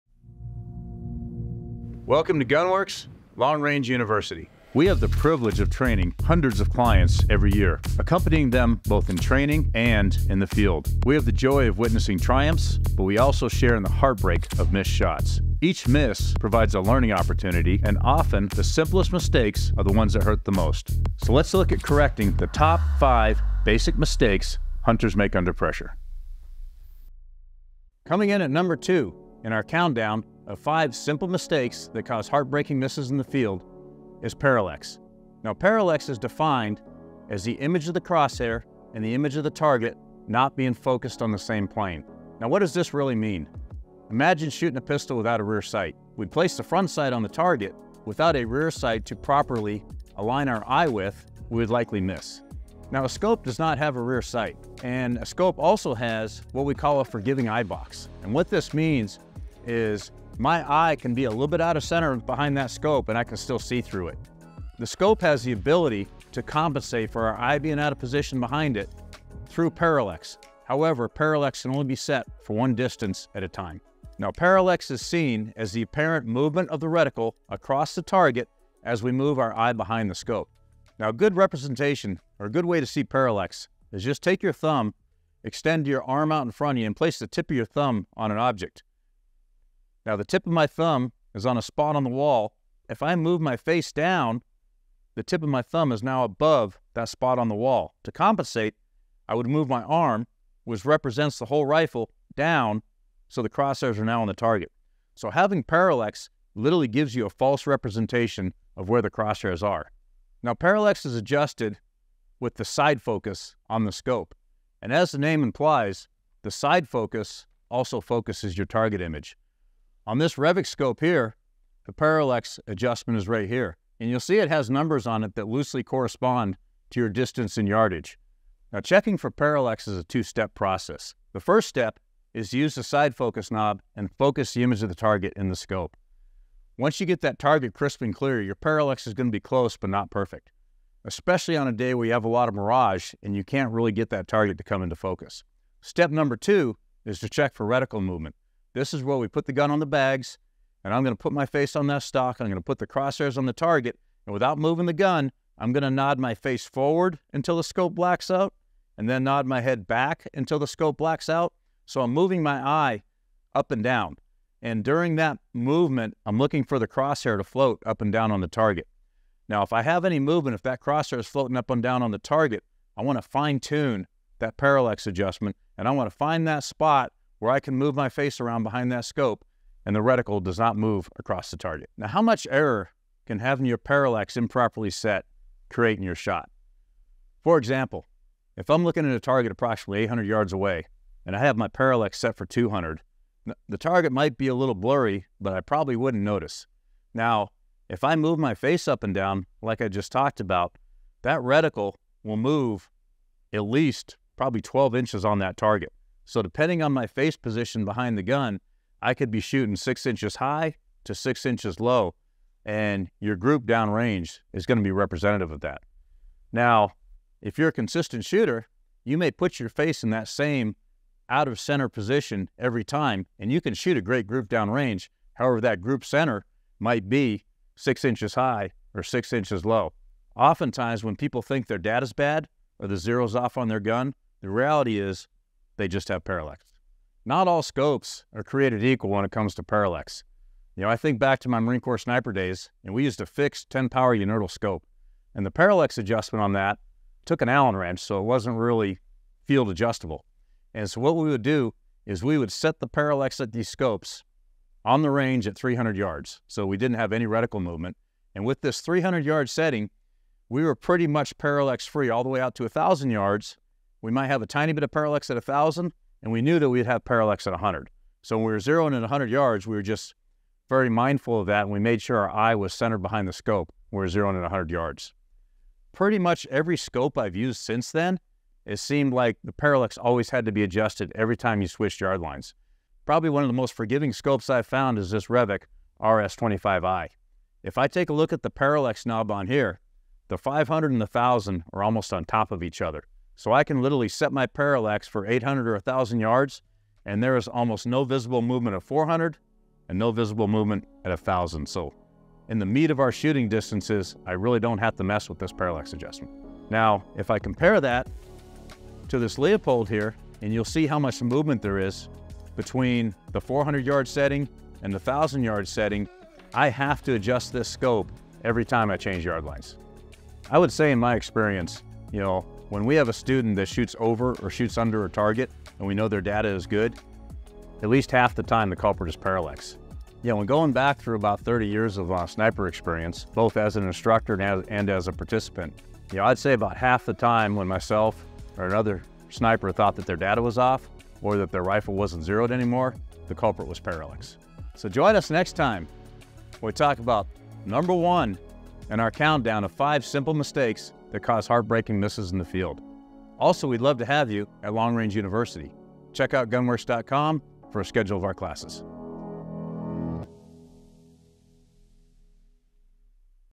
Top 5 Reasons We Miss Series | 2 | Improper Parallax
How does parallax hurt you? If your eye is out of center when parallax is present, the image reticle will, in reality, not be aligned with the image of the target. You will adjust the rifle to the image of the reticle, thereby aiming the rifle off the true center of your target.